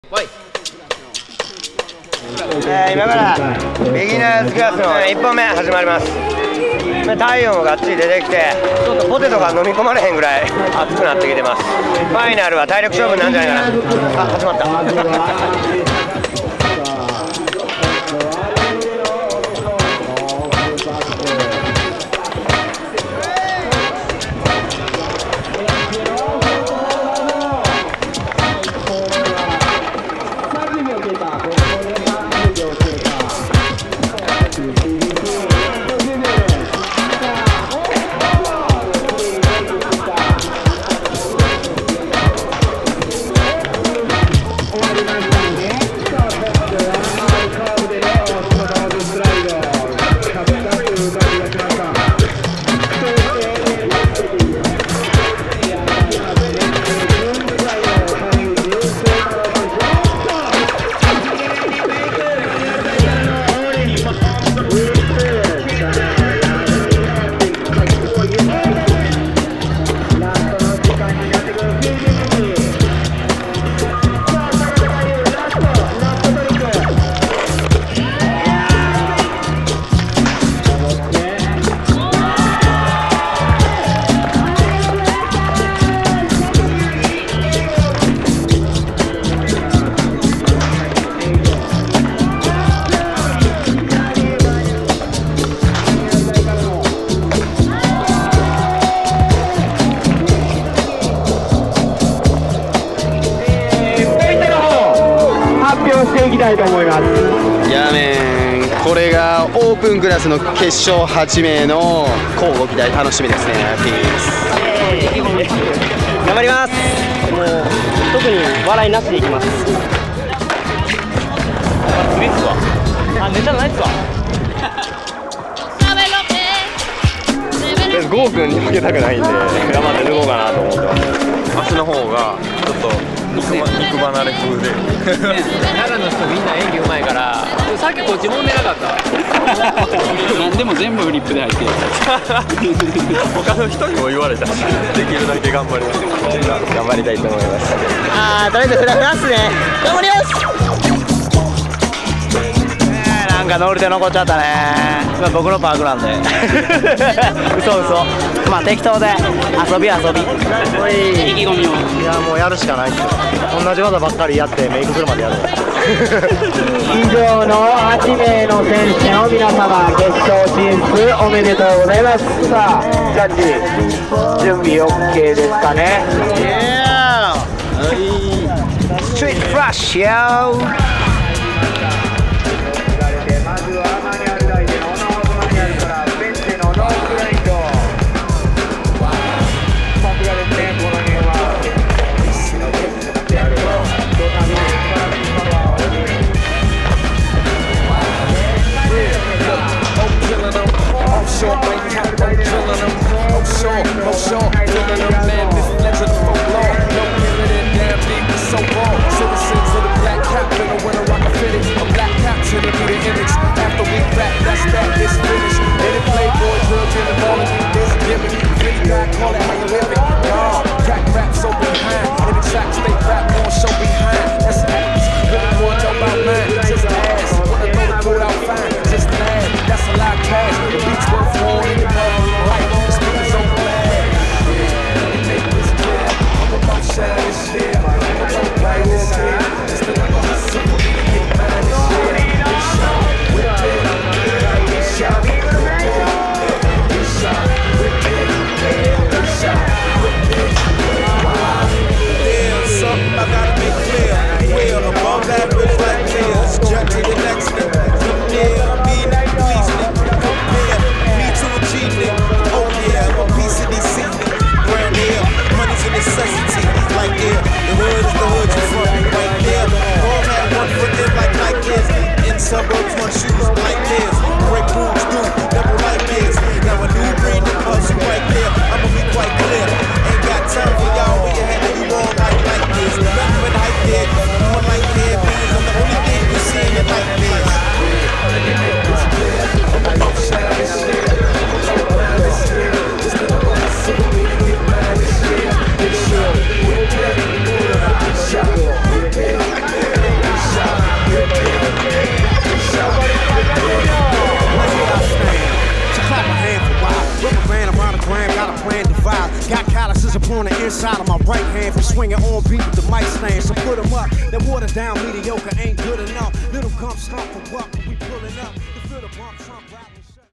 2009.5.31 STREET FLUSH 3/4
Nobutoshi Ito presents "STREET FLUSH" contest at Kyoryu skatepark.
Contents are Biginners class final and open class final 1st run.